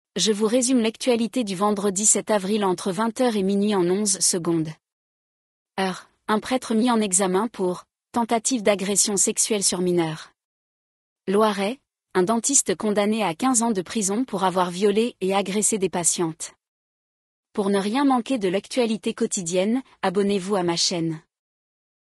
Résumé de l'actualité du jour : Vendredi 07 avril entre 20h et minuit
- Eure : un prêtre mis en examen pour « tentatives d’agressions sexuelles sur mineur »
- Loiret : un dentiste condamné à 15 ans de prison pour avoir violé et agressé des patientes